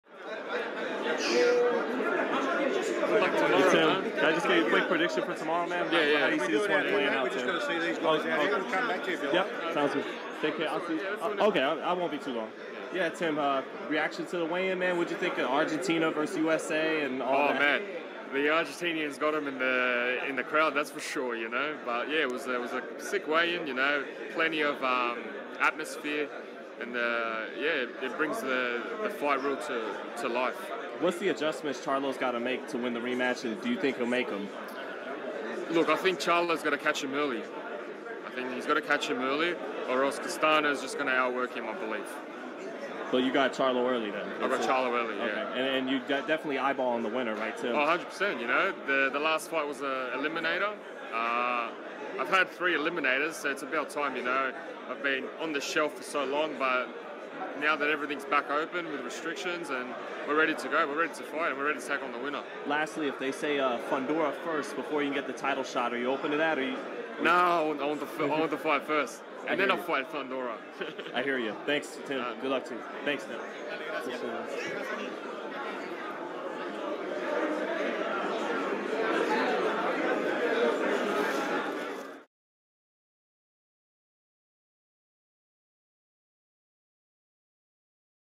TIM TSZYU FINAL CHARLO VS. CASTANO 2 PREDICTION; REACTS TO FIERY WEIGH-IN & WANTS WINNER
It's fight week! Unified WBC, WBA, & IBF jr. middleweight champion Jermell Charlo and WBO jr. middleweight champion Brian Castano face off in their highly-anticipated May 14 undisputed showdown at Dignity Health Sports Park in Carson, California. You don't want to miss what went down before, during, and after the fight. Check it out!